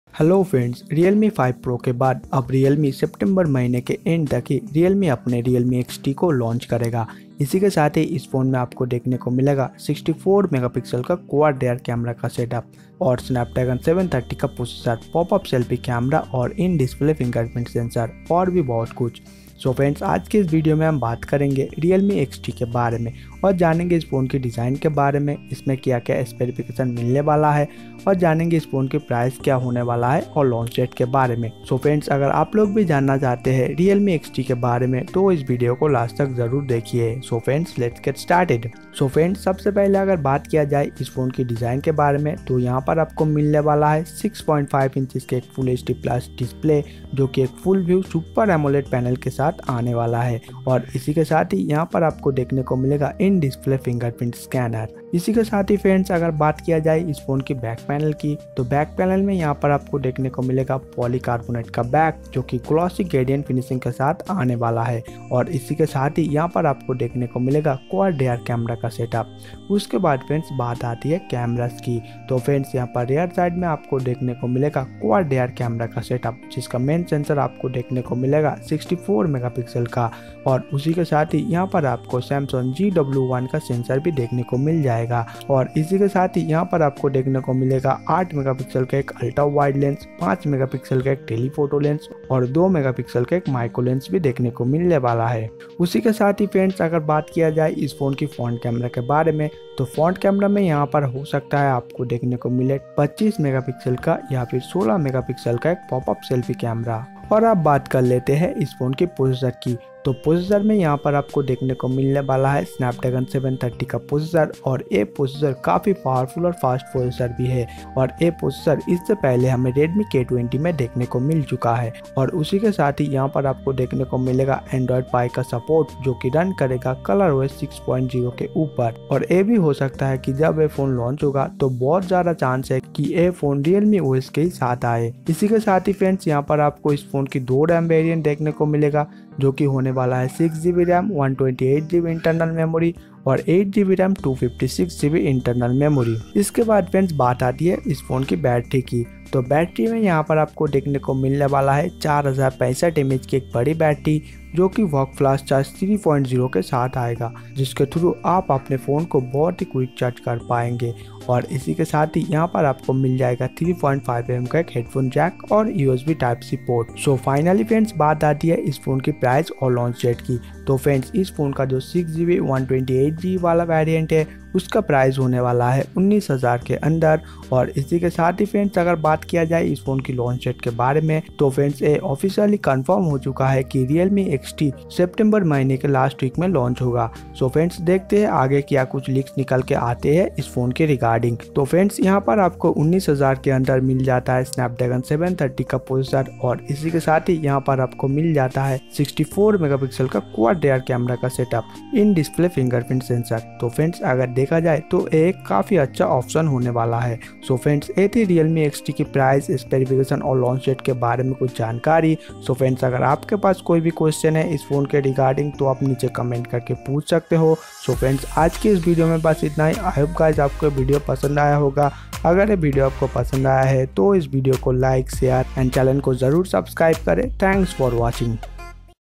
Realme XT - 64MP Quad Camera, Snapdragon 730, PopUp Selfie | Realme XT 64MPQuadCamera
It will come with snapdragon 730  processor  and quad  rear  camera setup. so friends if waiting for buying smartphone.
You can watch our alternative videos;-
Tap The Link Below To Watch ;-